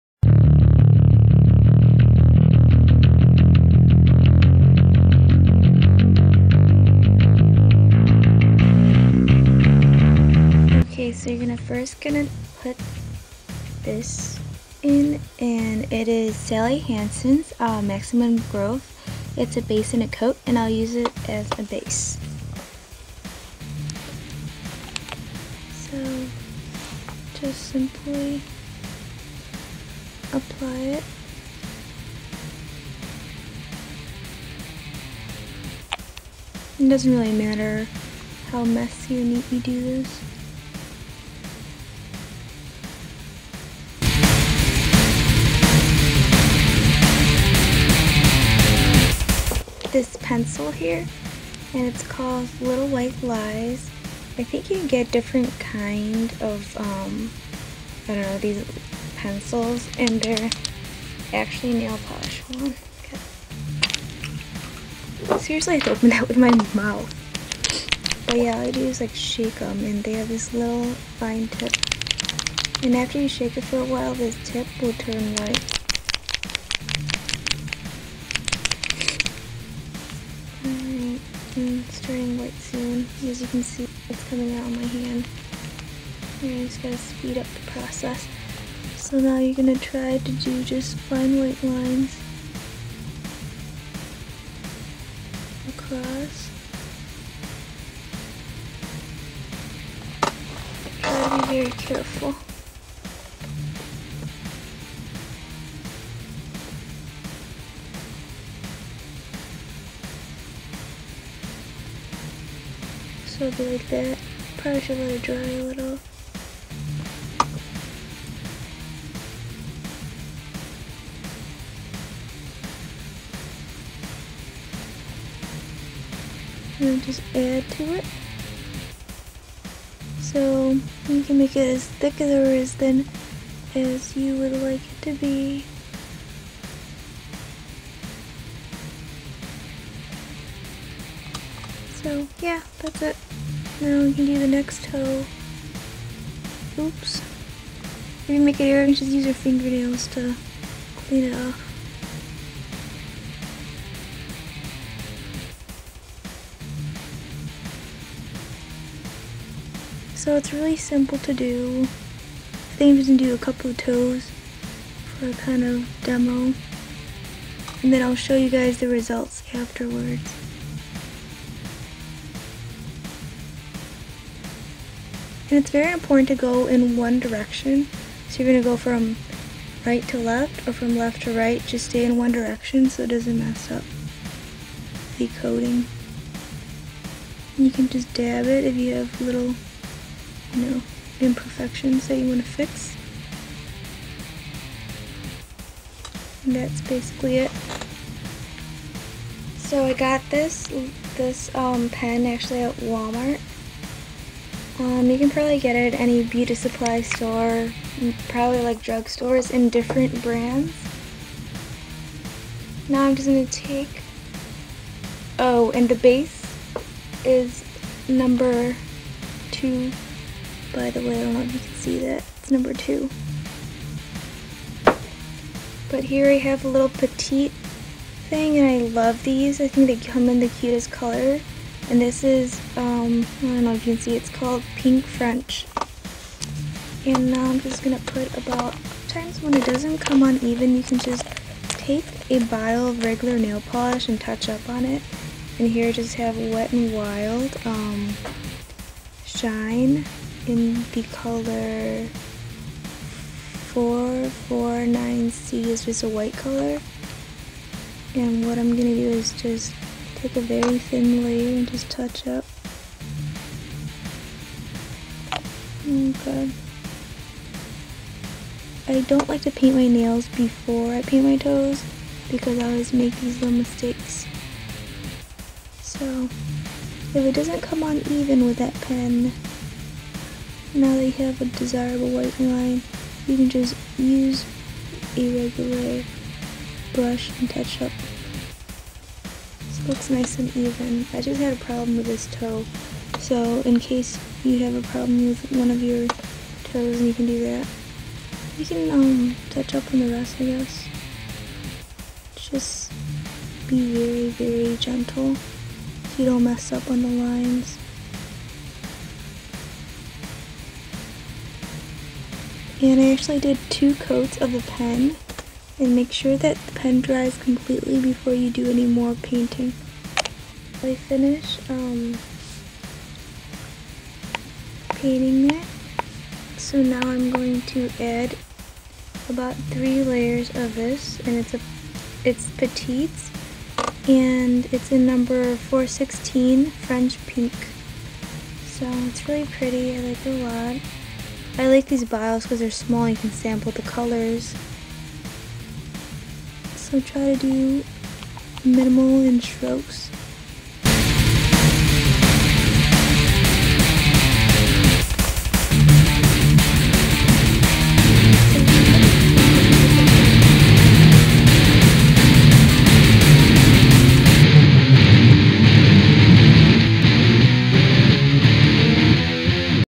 How Hanh french paints her toes
Items Used:

Sally Hansens Maxium growth top and bottom coat #02

Hoof Little White Lines Pen

Petite Pink French 416

Wet n Wild Shine 449C